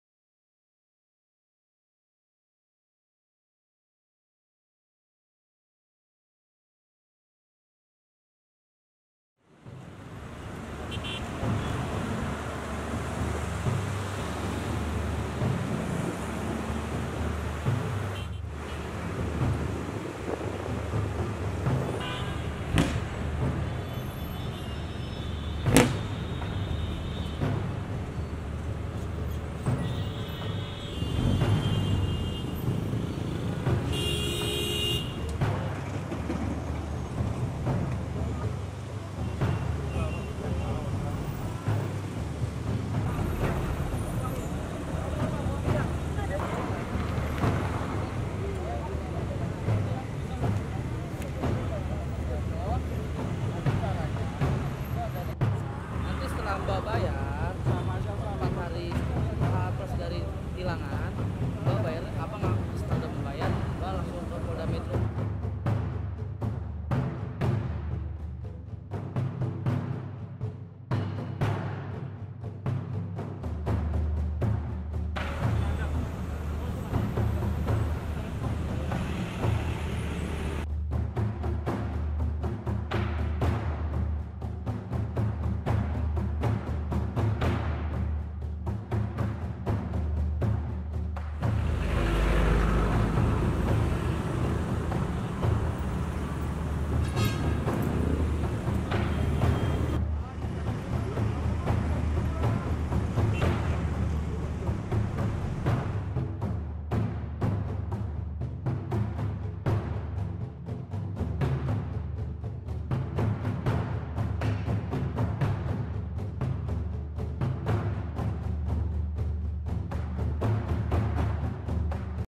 Polisi Merazia Pengendara Motor yang Menerobos JLNT Kampung Melayu-Tanah Abang
JAKARTA, NETRALNEWS.COM - Petugas Satuan Ketertiban dan Kelancaran (Tibcar) Lalu Lintas Polda Metro Jaya melakukan razia di Jalan Layang Nontol (JLNT) Kampung Melayu - Tanah Abang, kawasan Casablanca, Jakarta Selatan, Rabu (26/7/2017).

Razia tersebut dilakukan agar JLNT tersebut steril dari kendaraan roda dua. Meski telah ada rambu larangan untuk melintasi JLNT tersebut, banyak pengendara sepeda motor nekat menerobos guna menghindari kemacetan yang kerap terjadi di kawasan Karet hingga Casablanca.

Pada razia kali ini polisi berhasil melakukan penindakan terhadap puluhan pengendara motor yang membandel melalui jalur tersebut. Bahkan, di antara mereka yang terlanjur masuk dan mengetahui ada razia, nekat memutar balik dan melawan arus untuk menghindari polisi.